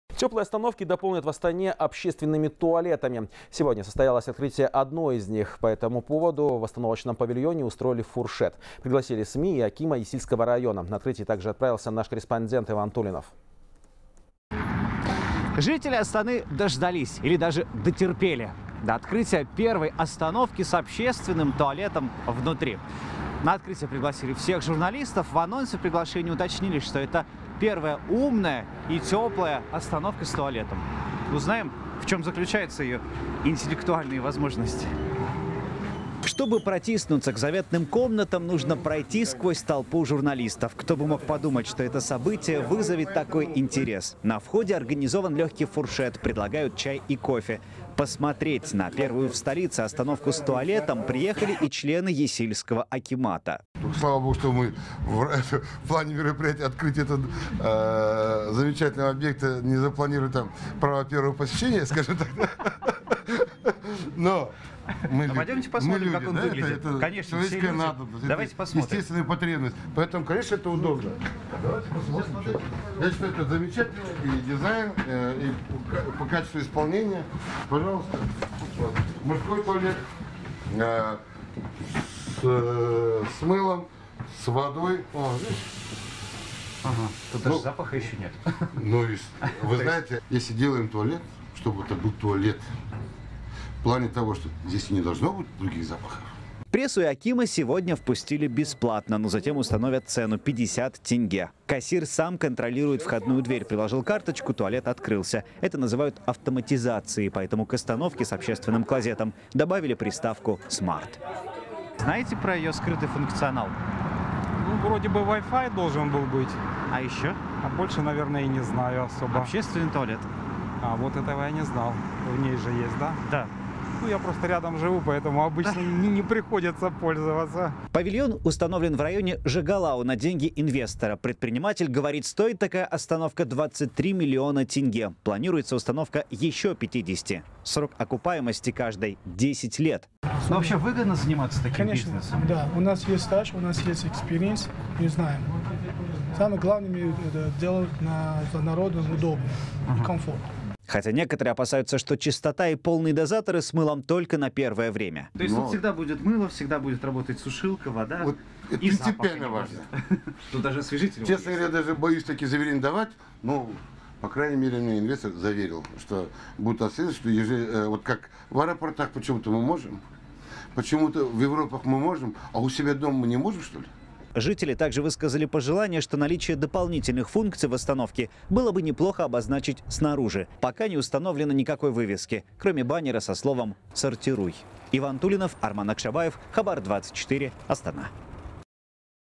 Теплые остановки в Астане дополнят туалетами
Тёплые остановки дополнят в Астане общественными туалетами, сообщает корреспондент «Хабар 24». Сегодня состоялось открытие одной из них. По этому поводу в остановочном павильоне устроили фуршет. Пригласили СМИ и акима Есильского района.